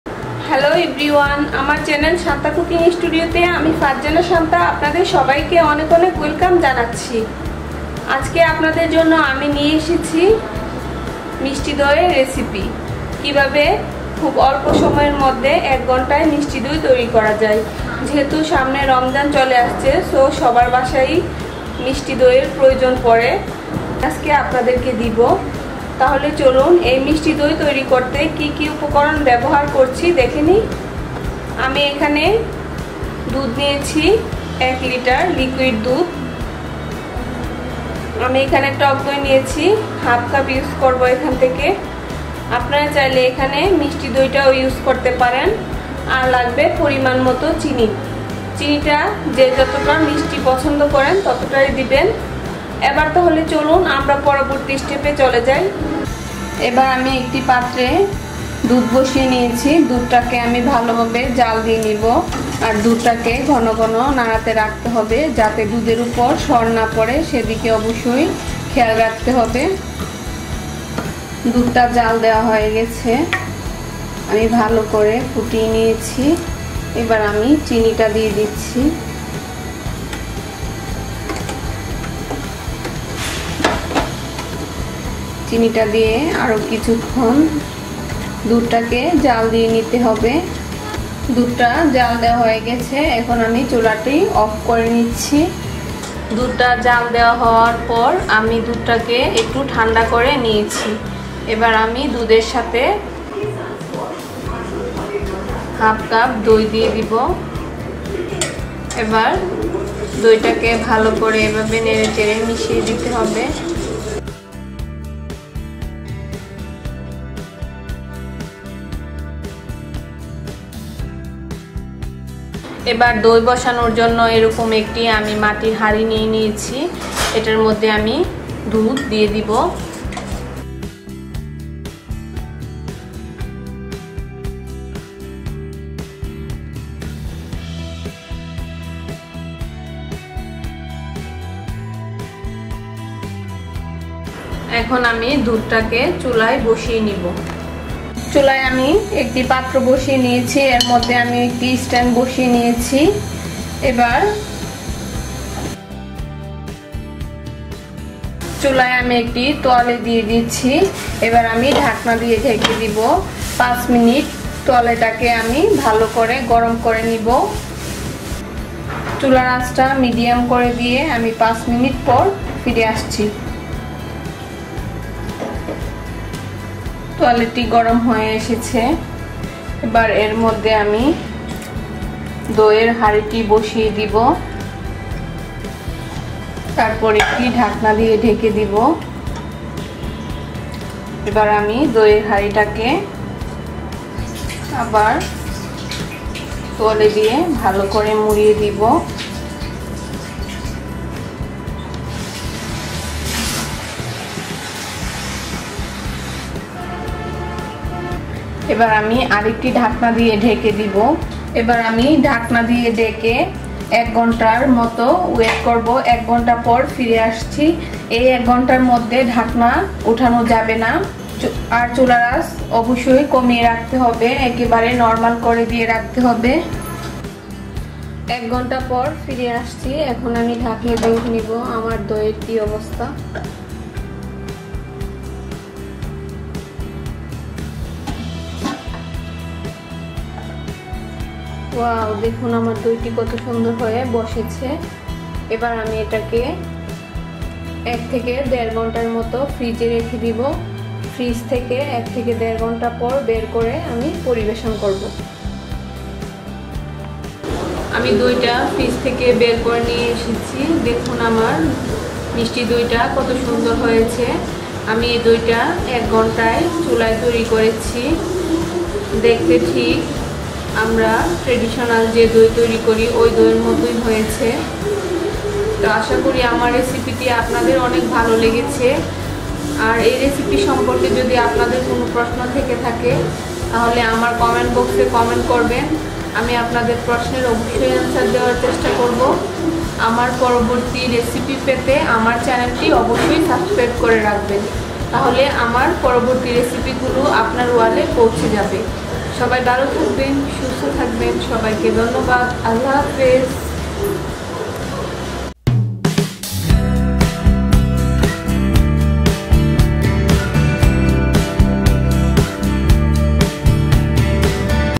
পারফেক্ট মিষ্টি দই/বিয়ে বাড়ীর দই/১ ঘন্টায় মিষ্টি দই তৈরির সহজ রেসিপি/How To Make Mishti Doi
বেশিরভাগ রাঁধুনিরই এই অভিযোগ থাকে যে দই জমে না। দই তৈরি করা একটু ঝামেলারই বটে। আপনারও কি একই সমস্যা? মাত্র ১ ঘণ্টায় দই তৈরির রেসিপি। সঠিক রেসিপি জানলে আসলে সব সম্ভব! তাই আমি আজ আপনাদের জন্য এই দইয়ের রেসিপি নিয়ে এসেছি তাহলে চলুন দেখেনেই এই রেসিপিটি.